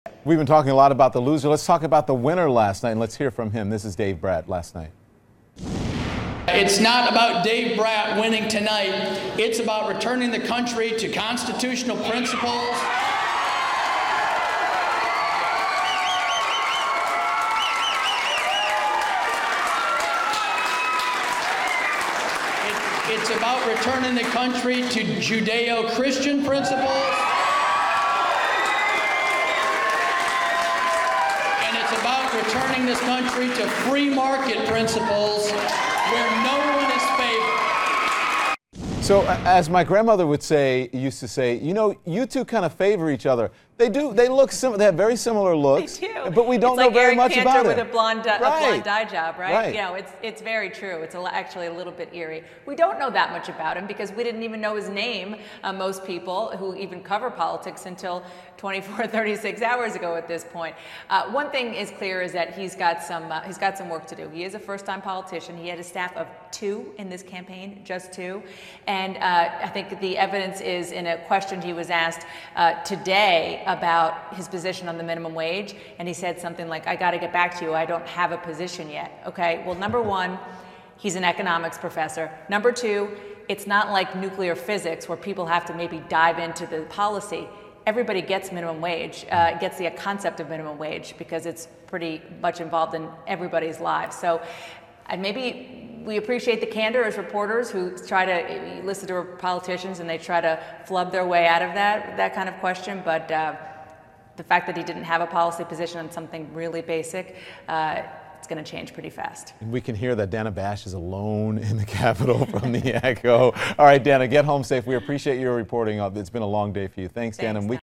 Who is Dave Brat?
Reporting from Washington, CNN's Dana Bash offers some insight and perspective on the economics professor who famously ousted heavy favorite Eric Cantor on Tuesday evening.